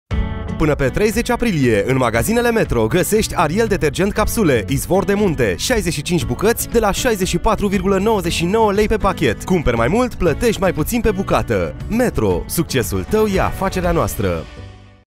La METRO cumperi mai mult, plătești mai puțin pentru detergent ARIEL capsule!
La METRO cumperi mai mult, plătești mai puțin și profiți din plin!

👉 Disponibil în perioada 1 - 30 Aprilie 2024

METRO. Succesul tău e afacerea noastră!